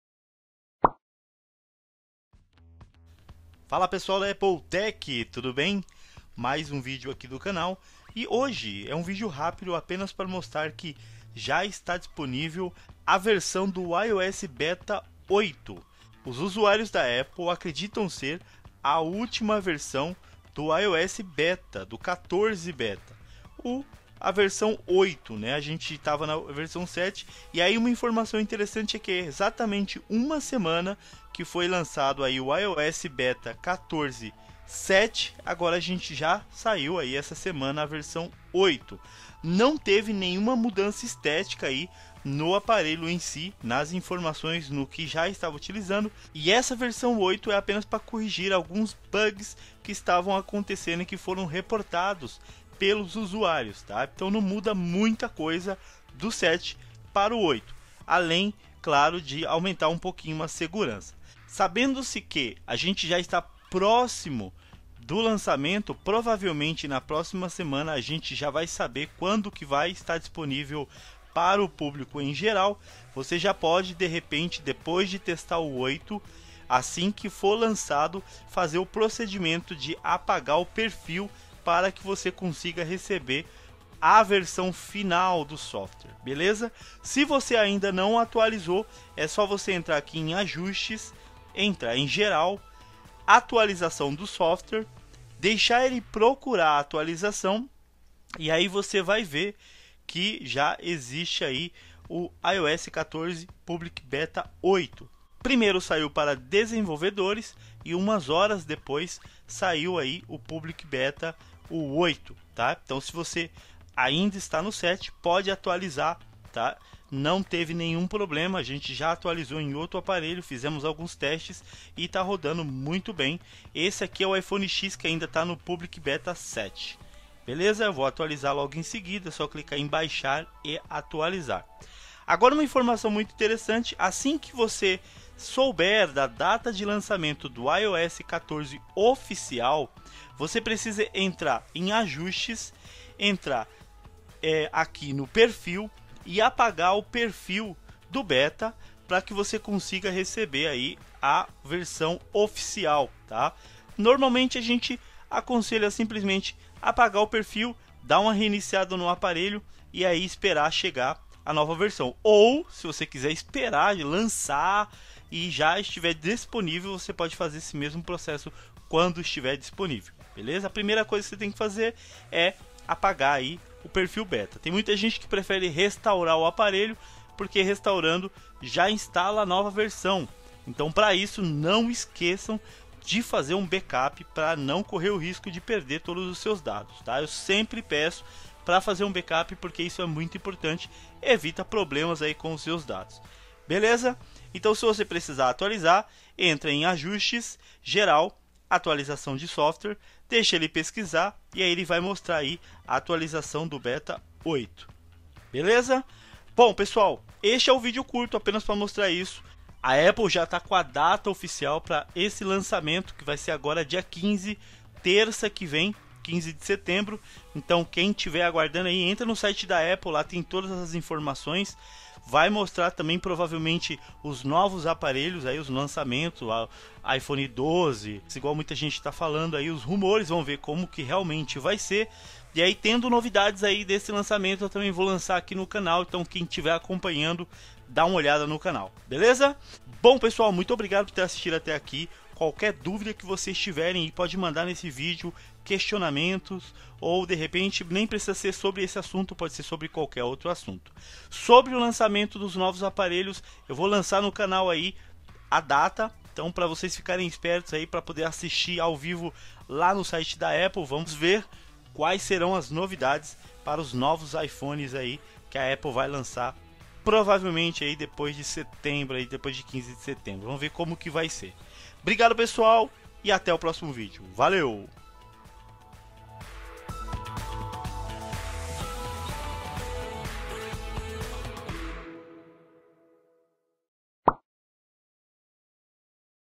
ÚLTIMA ATUALIZAÇÃO DO IOS 14 BETA - IOS 14 BETA 8 - APPLE SUPORTE
Galera nesse momento difícil quero pedir a todos que fiquem em casa e lave bem as mãos, quem não puder use mascara. Obrigado

Guys in this difficult time I want to ask everyone to stay at home and wash your hands.Thanks

DESCRIÇÃO :
Neste vídeo mostro a nova atualização do IOS beta 8, provavelmente a ultima antes do lançamento da versão oficial, que esta programado para ser anunciado dia 15 de setembro.

Aproveite esse vídeo e deixe seu comentário ou sua pergunta.

Abraços